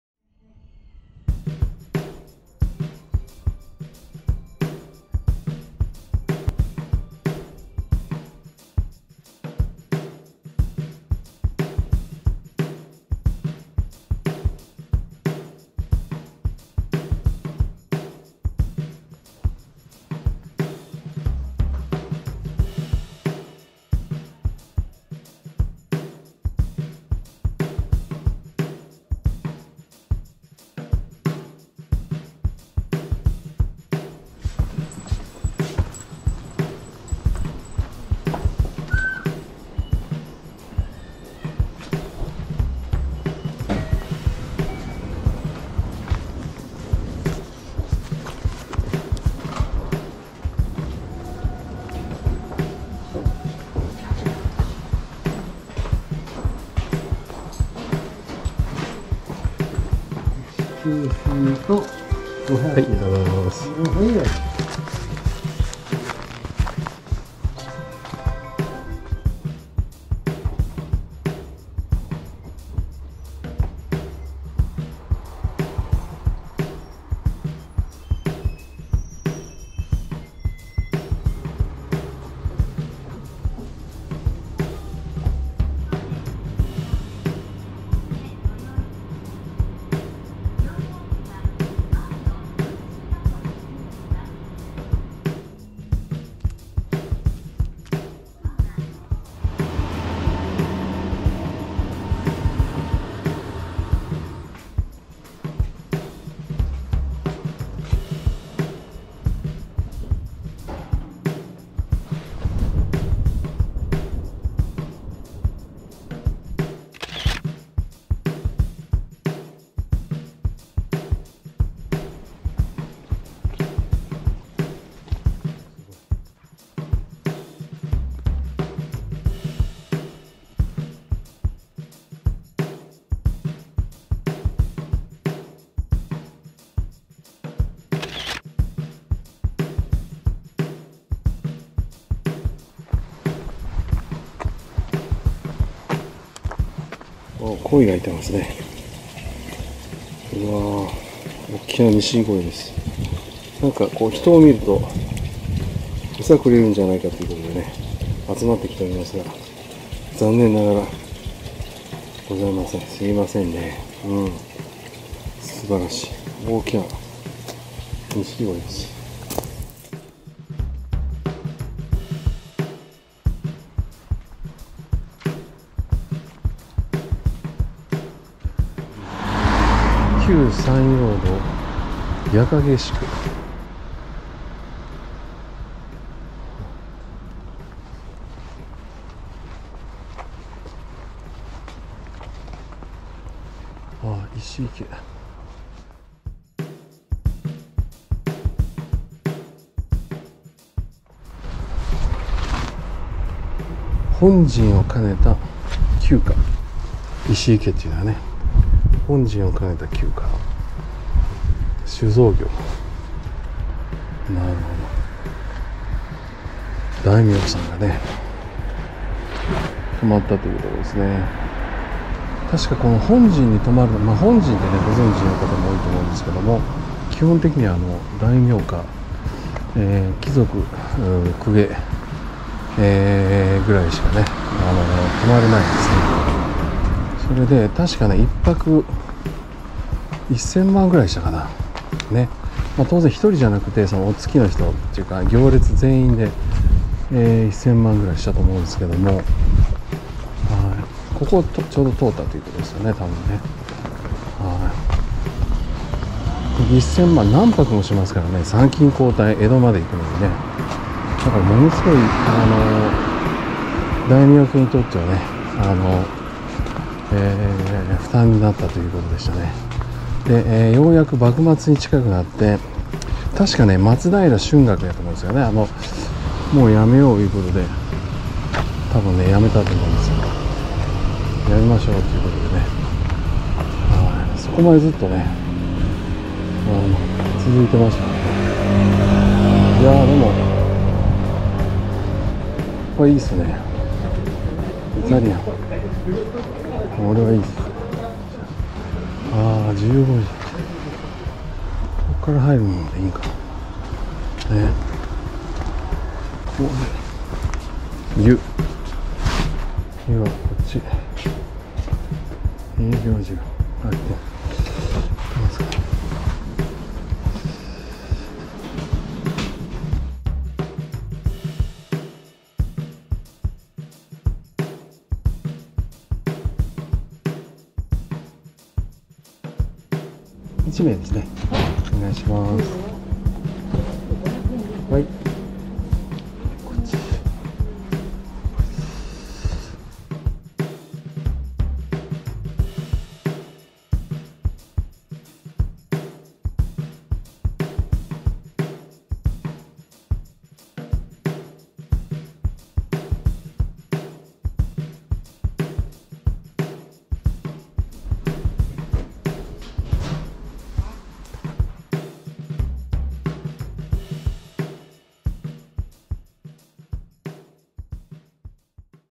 2021.2[井原鉄道･矢掛駅]日帰り入浴[矢掛宿]
旧山陽道･矢掛宿にある日帰り入浴を利用しました。